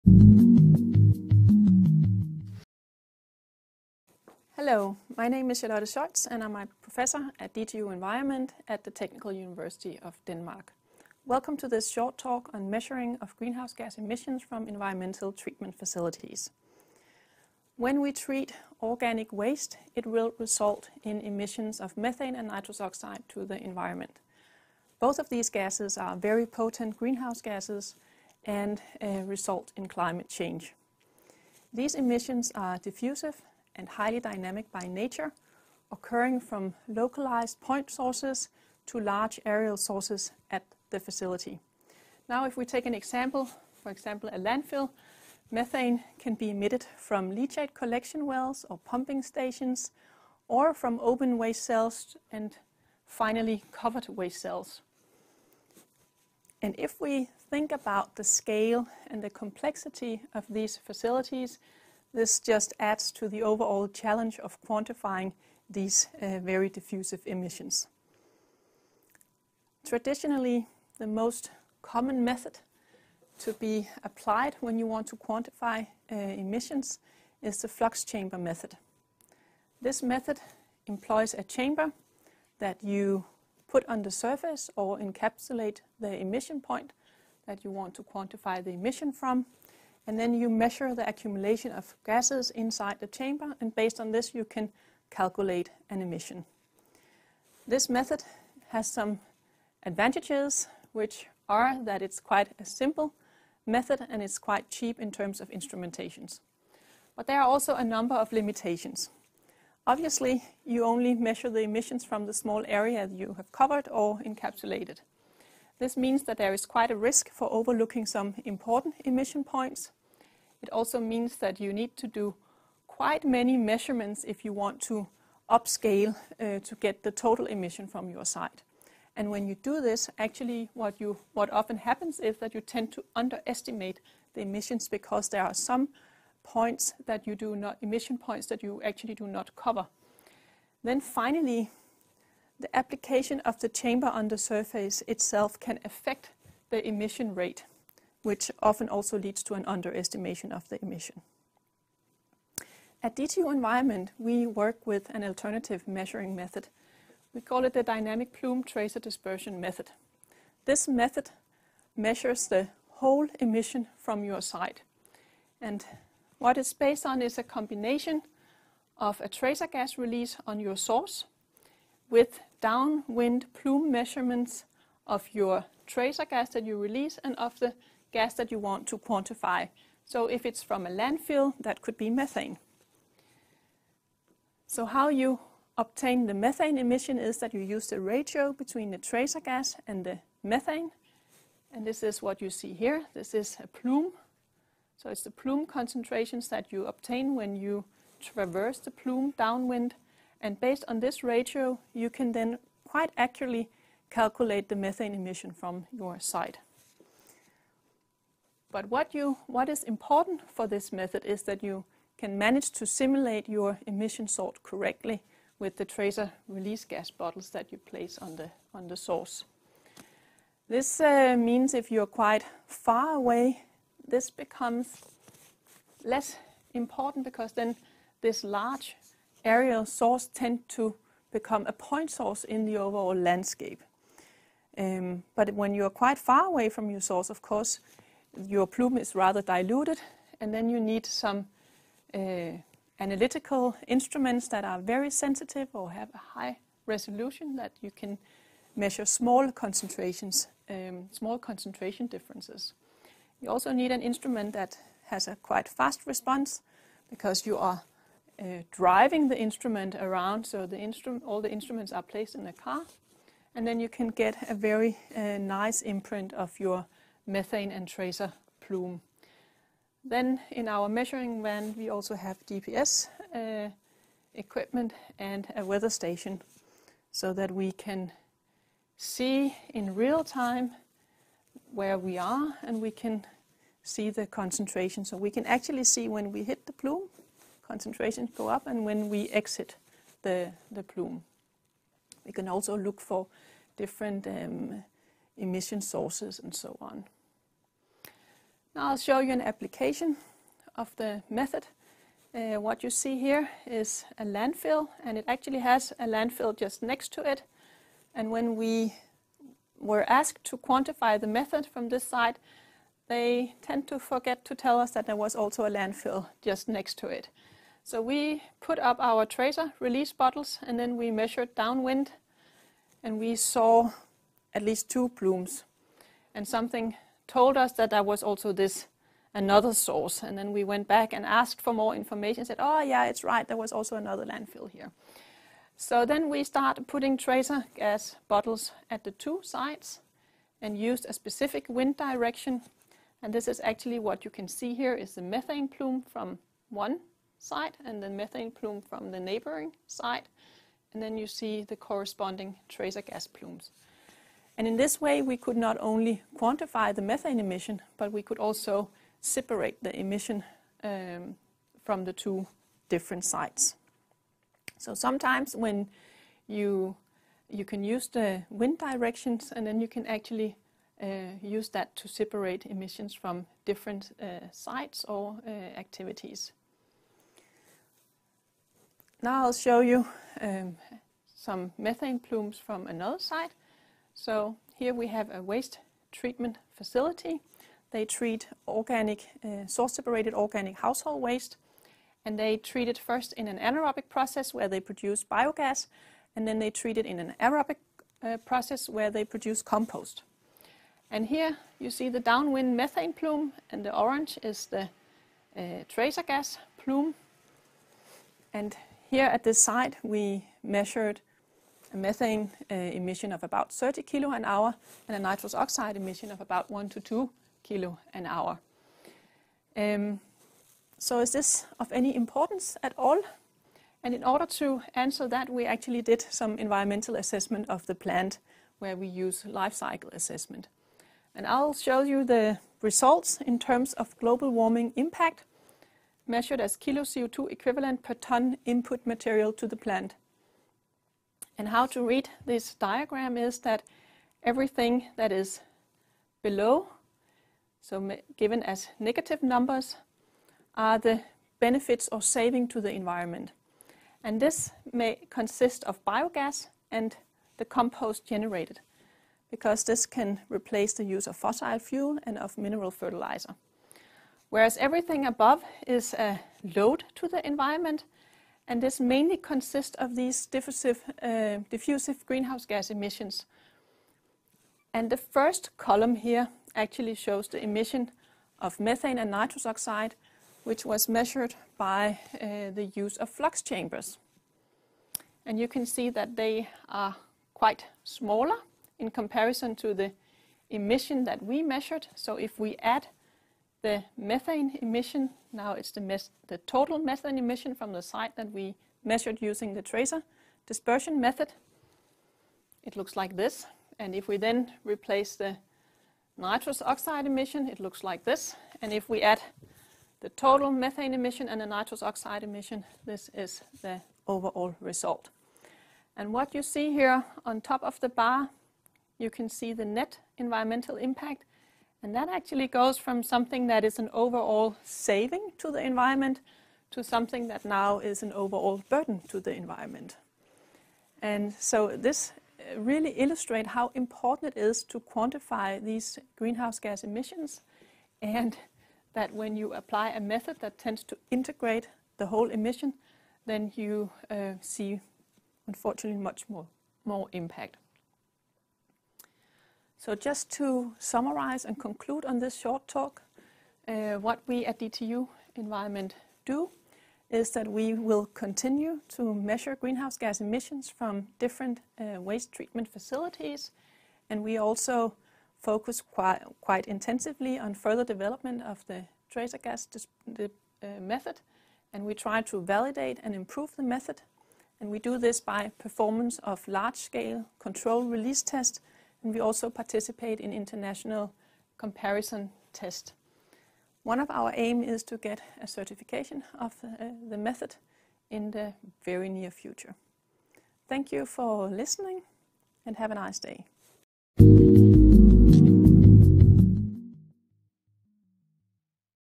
Measuring Greenhouse Gas Emissions from Environmental Facilities
By Charlotte Scheutz, Professor, DTU Environment